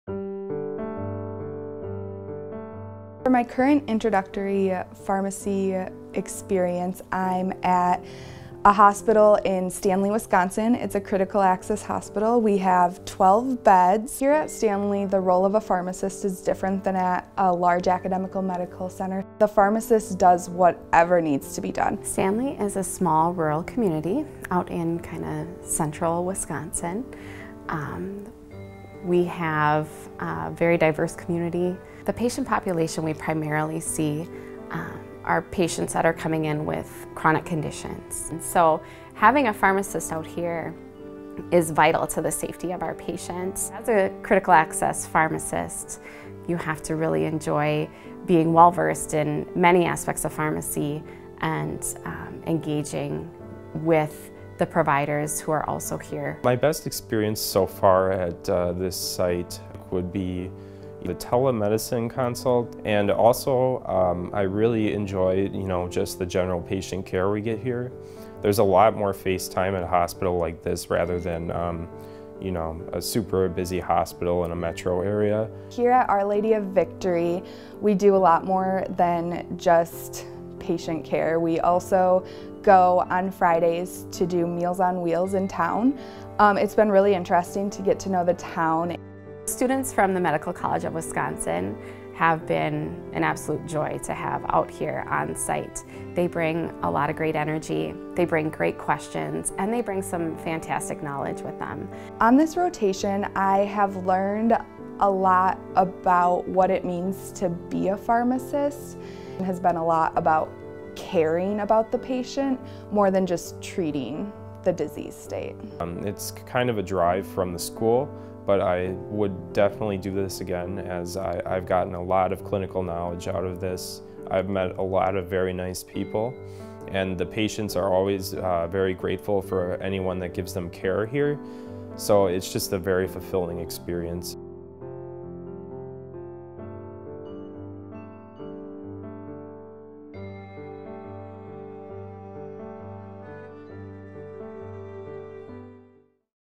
Rural health rotation at the MCW School of Pharmacy
The MCW School of Pharmacy offers a rural health Introductory Pharmacy Practice Experience (IPPE) rotation at Ascension Our Lady of Victory Hospital in Stanley, WI as part of its aim to address the growing need for primary care services in medically underserved areas.  Students gain unique experiences in areas such as telehealth.